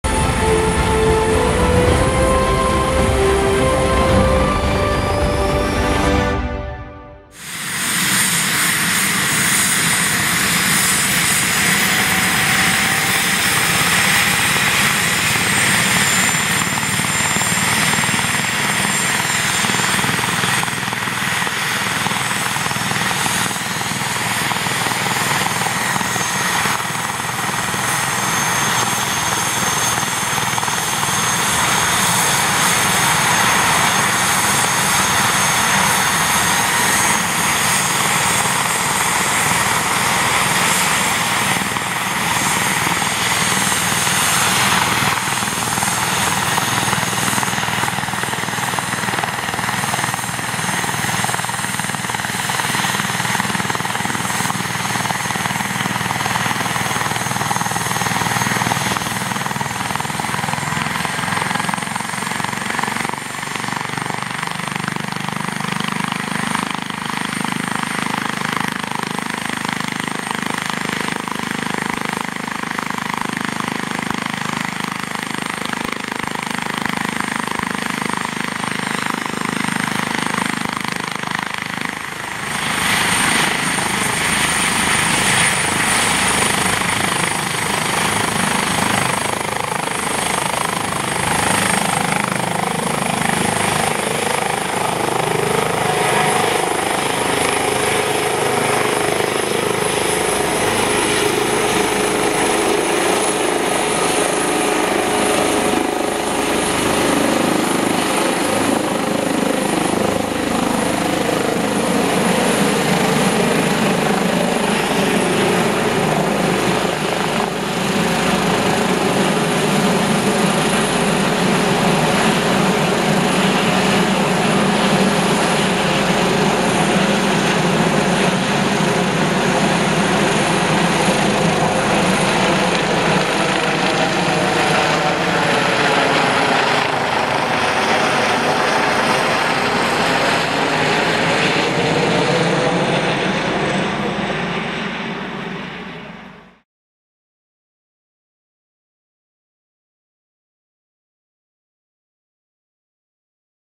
[Start auf Feld] Christoph Sachsen-Anhalt Start in Halle/ Dölau
Nachtrag vom Juni/2021

Christoph Sachsen-Anhalt / DRF Luftrettung / H145~EC-145T2~BK-117D2 / Halle/ Dölau / D-HILF